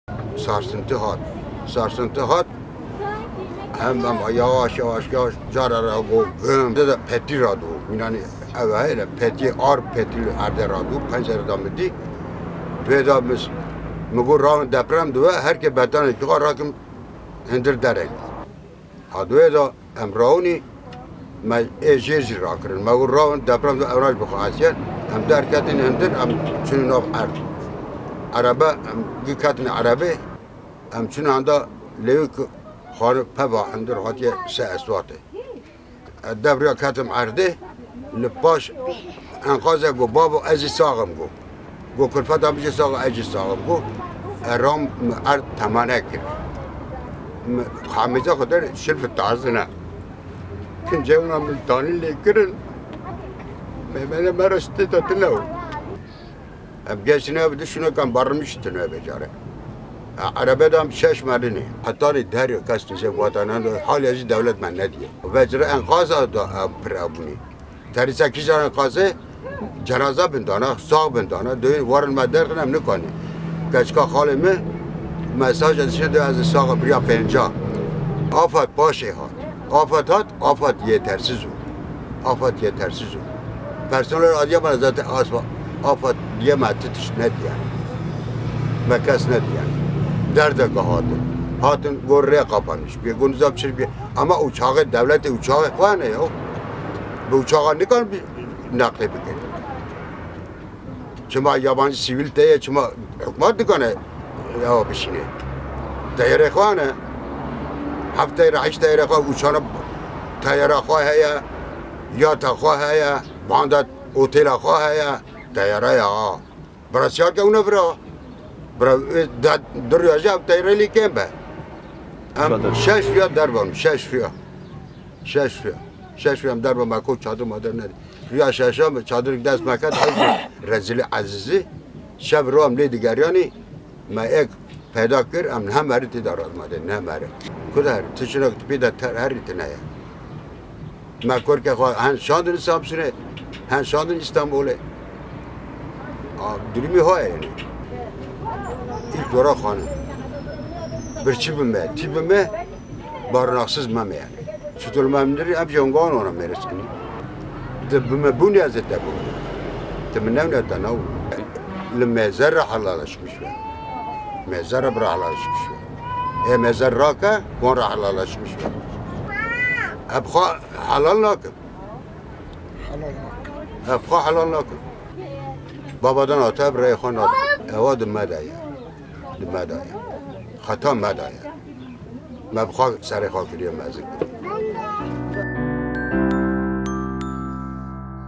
LI SEMSÛRÊ MEXDÛREK ERDHEJÊ: HEQÊ XWE HELAL NAKIM DENGÊ XWE NADIM DESTHILATÊ
Maxdûrê erdhejê Emîn Durmaz ku li navenda Semsûrê di konbajar de dijî bi lêv kir ku bi rojan alîkariya wan nehatiye kirin û wiha bertek nîşan da: "Ez bi xwe heqê xwe helal nakim. Lê me wan li ser serê xwe mezin kir, heke me deng nedana wisa nedibû. Êdî divê kes dengê xwe nede desthilatê."